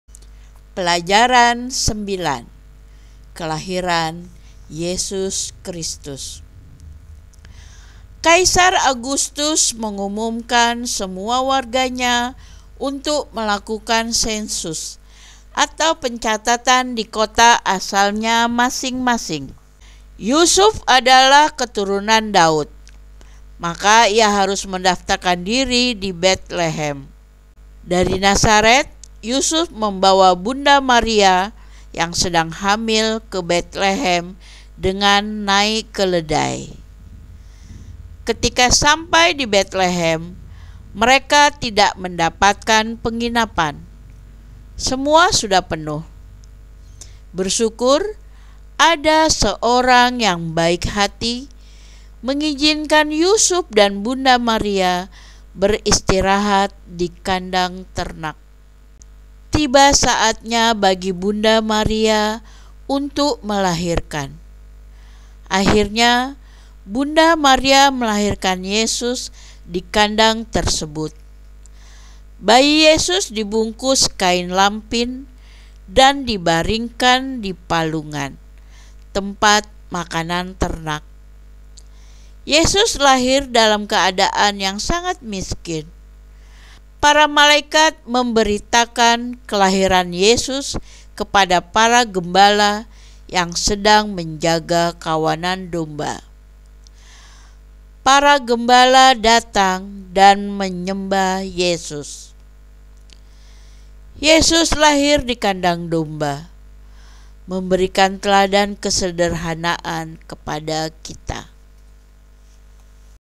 Kelahiran Yesus Kristus Pelajaran IX Pendidikan Iman Anak Kelas II
Sensus penduduk yang diwajikan oleh pemerintahan Kaisar Agustus, memaksa Yusuf dan isterinya Maria, yang sedang mengandung harus berangkat dari Nazaret menuju Betlehem.
Ketika tiba di Betlehem sudah tiba waktunya bagi Maria untuk melahirkan. Tetapi mereka tidak menemukan tempat penginapan, sehingga akhirnya mereka beristirahat di sebuah kandang domba.  Maria melahirkan bayi Yesus disana. Berita kelahiran Yesus disampaikan para malaikat kepada para gembala di sekitarnya. Sehingga para gembala datang untuk melihat langsung bayi Yesus tersebut.